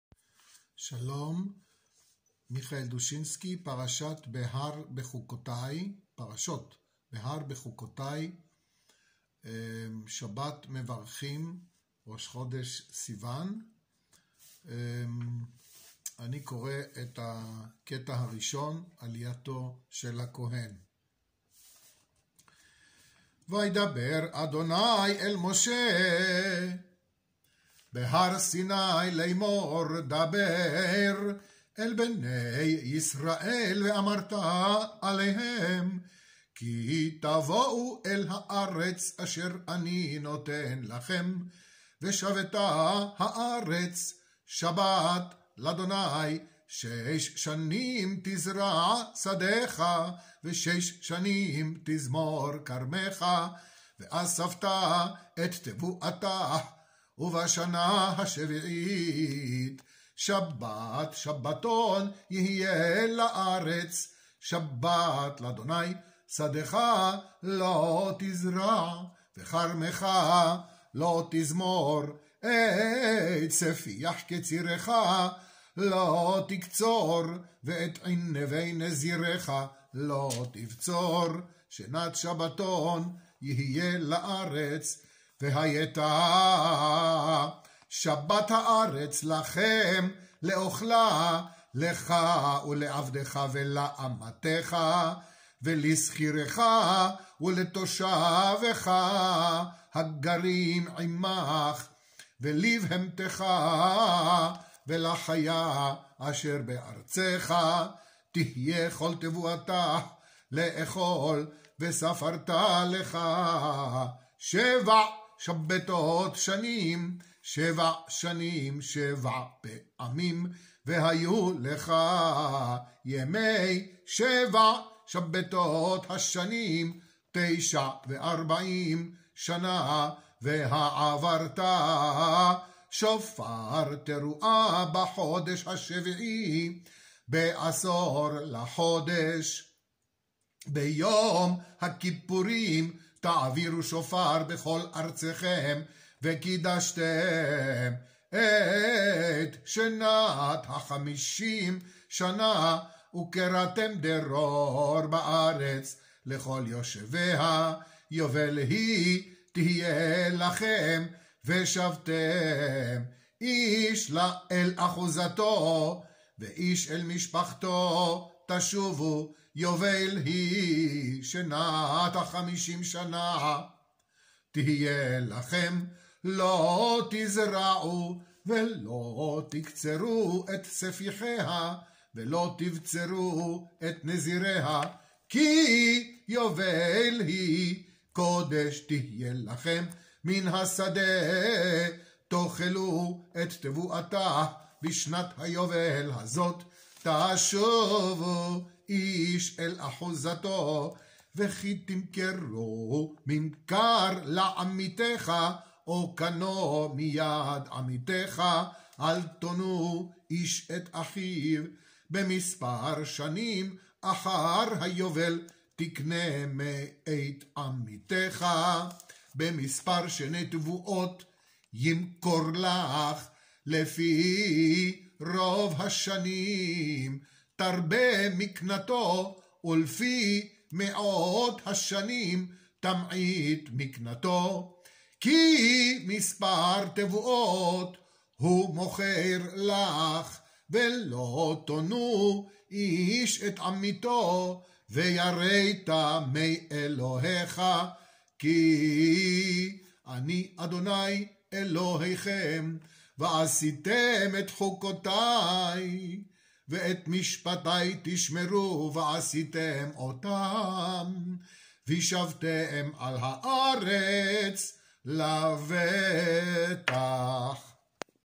פרשת בהר-בחוקותי_1 תש''ף הרב מיכאל דושינסקי Rabbi Michael Dushinsky Parashot Behar-BeChoqotai_1 2020
הרב מיכאל דושינסקי - הפקות Rabbi Michael Dushinsky - Productions
ב"ימי קוֹרוֹנה" - אייר תש"ף, בפראג, מיכאל דושינסקי קורא עליית כהן, "ראשון", של פרשות בְּהַר-בְּחֻקֹּתַי, בטעמי אשכנז בנעימה של "פולין-ליטא", מזרח-אירופה: ויקרא כה א-יח. שבת שלום ובריאות שלמה! אמן
 In “Corona Days”, May 2020, in Prague, Rabbi Michael Dushinsky reads the 1st ‘Aliya - of Kohen, "Rishon", of Parashot: BeHar-BeChuqotai, in Ashkenazi ‘Trop’ of the “Poland-Lithuania”, East-European melody: Vayiqra\Leviticus chapter 25 verses 1-18. Shabbat Shalom and Complete Health! Amen
Prague 1,
Czech Republic

Links